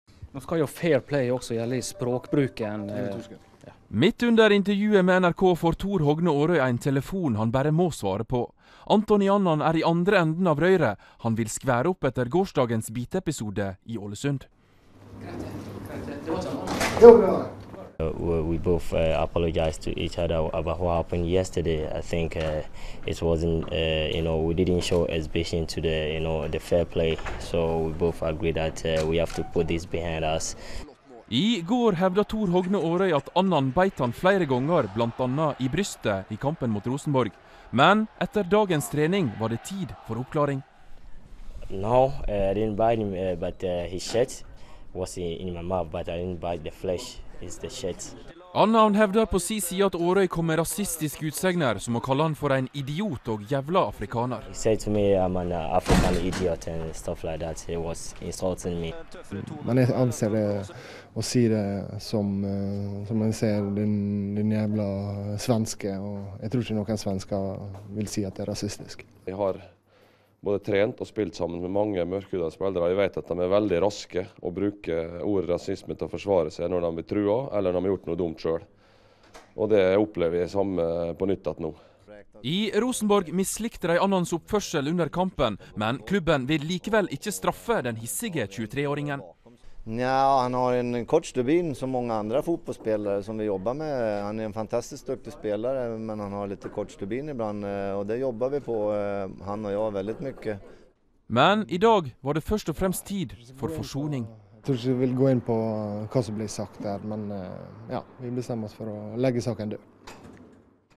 Ghana's Anthony Annan denies biting Norwegian player Aaroy
Ghana and Rosenborg midfielder Anthony Annan explains what happened between him and Aalesund striker Aaroy. Annan insists he didn't bite the player and their beef has ended.